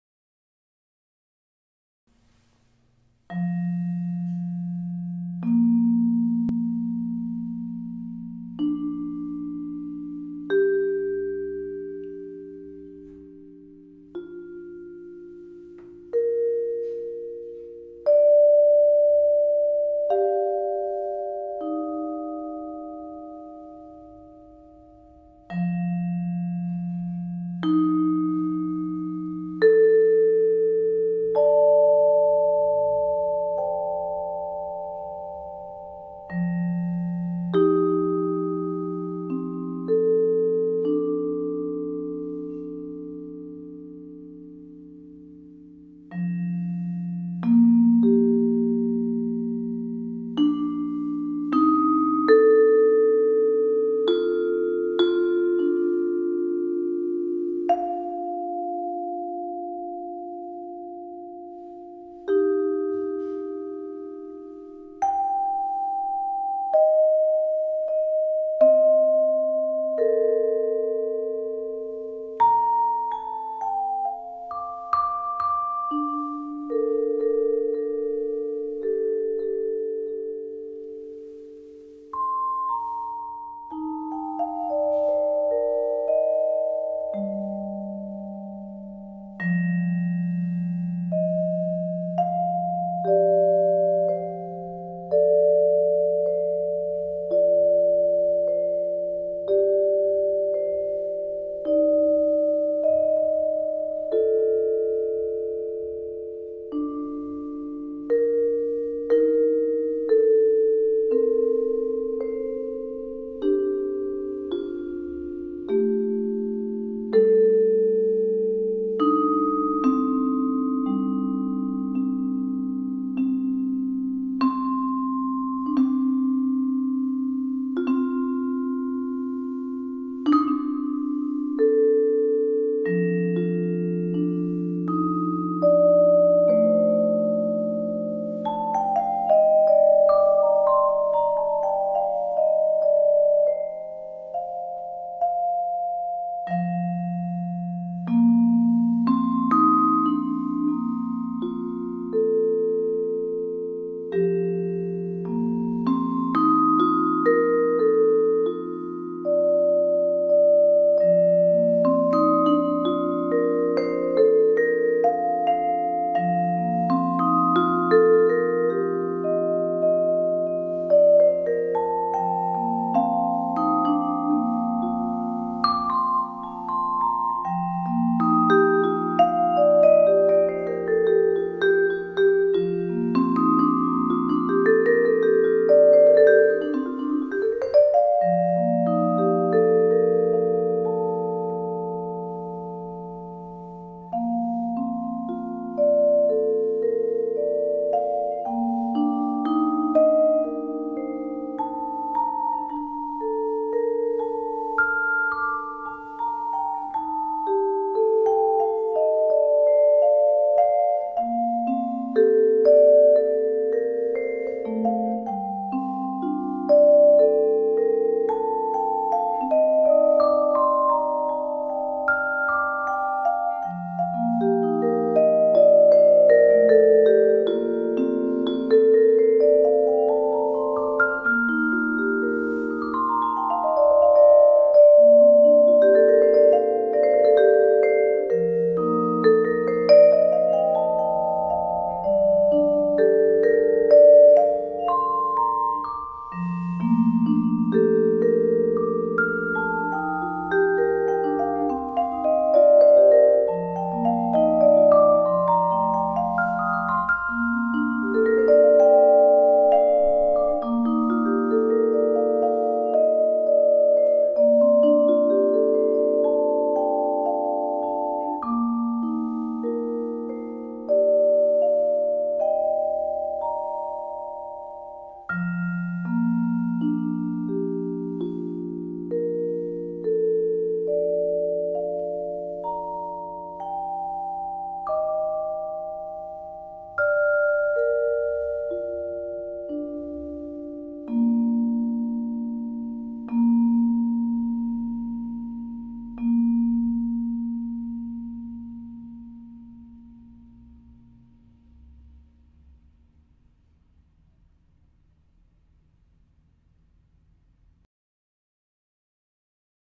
Daily Improvisation #55 - 1 Vibraphone / 2 Mallets / Bb-Major (Julius Heise)
No. 55 of my daily improvisations & No. 6/12 in my 2-Mallet-tunes dedicated to one ionic scale each.

Vibraphone & InstantComposition - Julius Heise